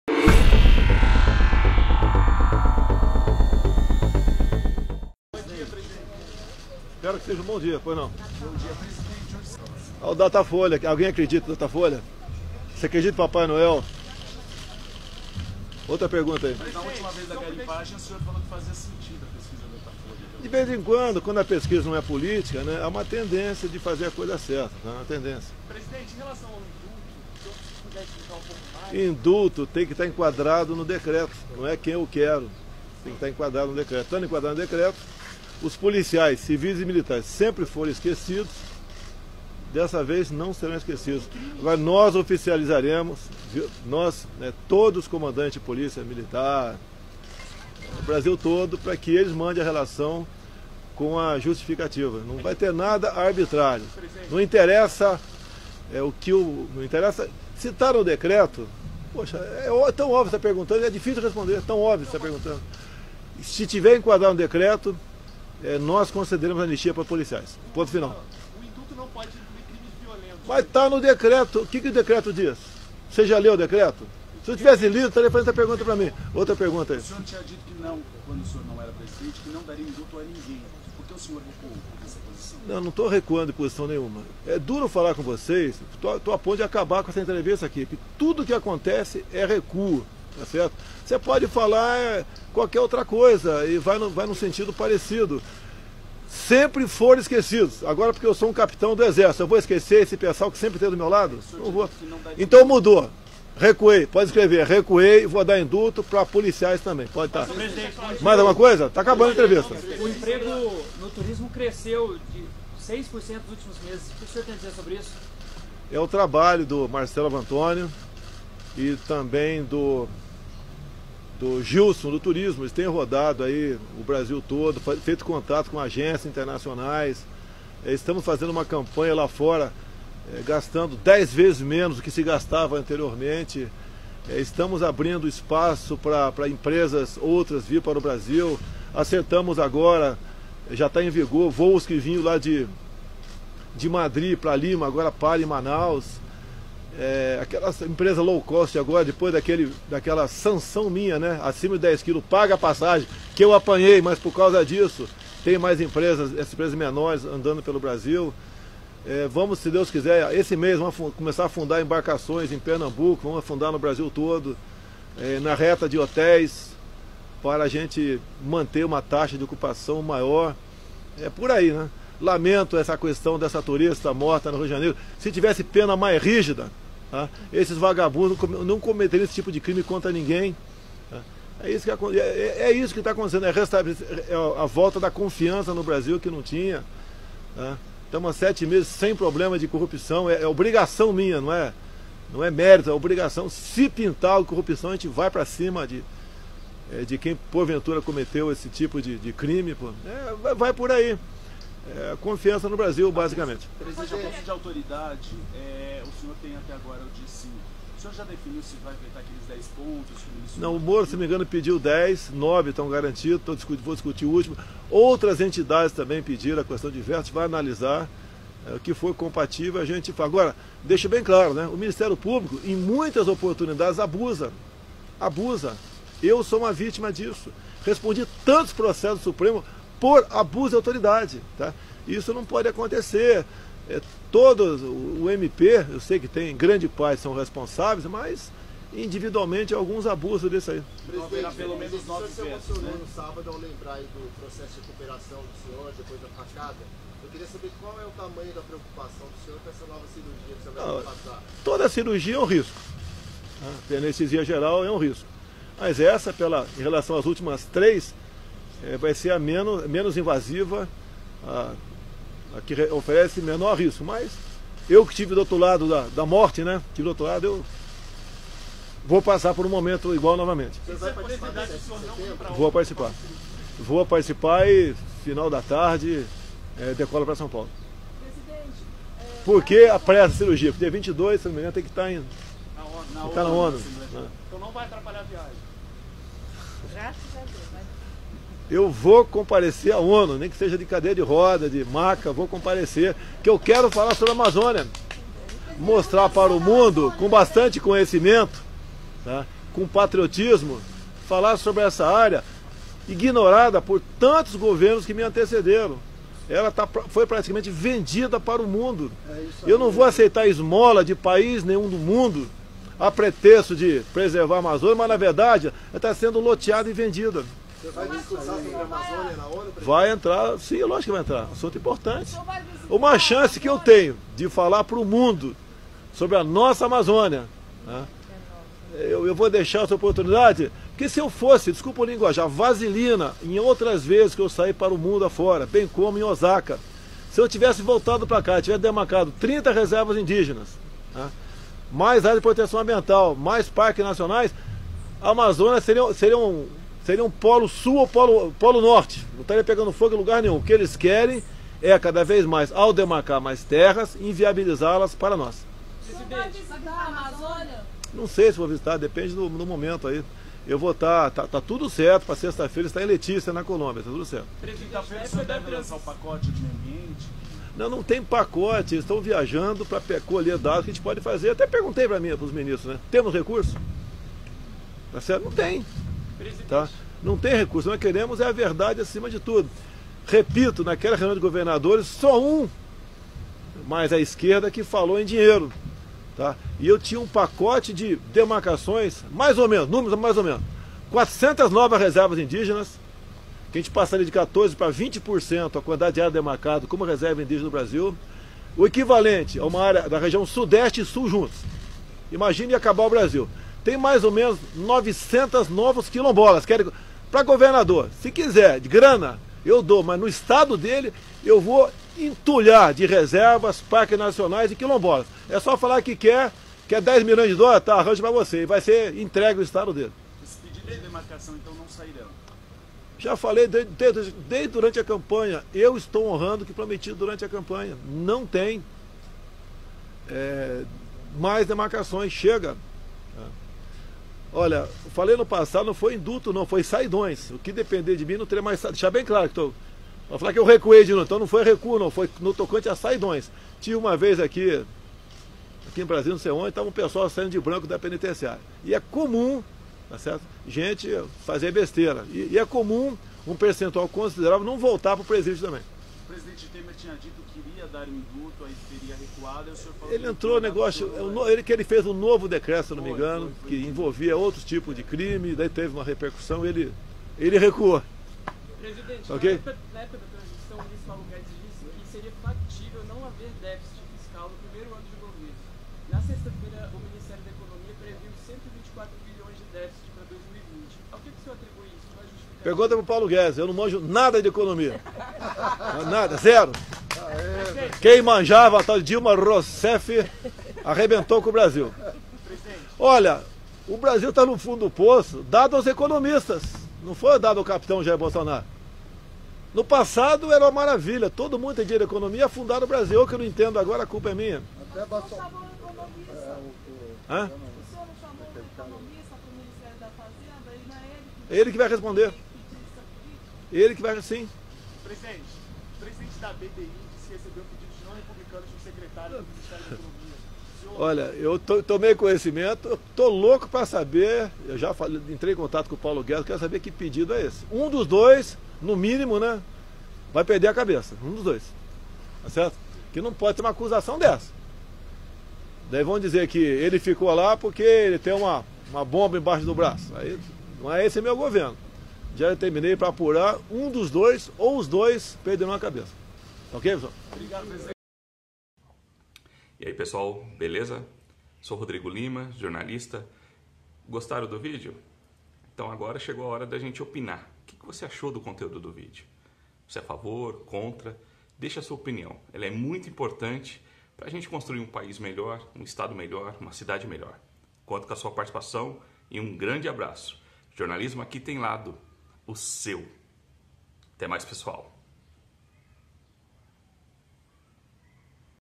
BOLSONARO DIZ QUE IRÁ À ONU 'NEM QUE SEJA DE CADEIRA DE RODAS' - Ele quer falar da Amazônia
O presidente Jair Bolsonaro (PSL) afirmou nesta segunda-feira, 2, que pretende ir fala da Amazônia na Organização das Nações Unidas (ONU) mesmo se estiver usado "cadeira de rodas". Ele irá passar por cirurgia no próximo domingo em São Paulo. Bolsonaro criticou ainda o Ministério Público ao comentar o veto a lei do abuso de autoridade.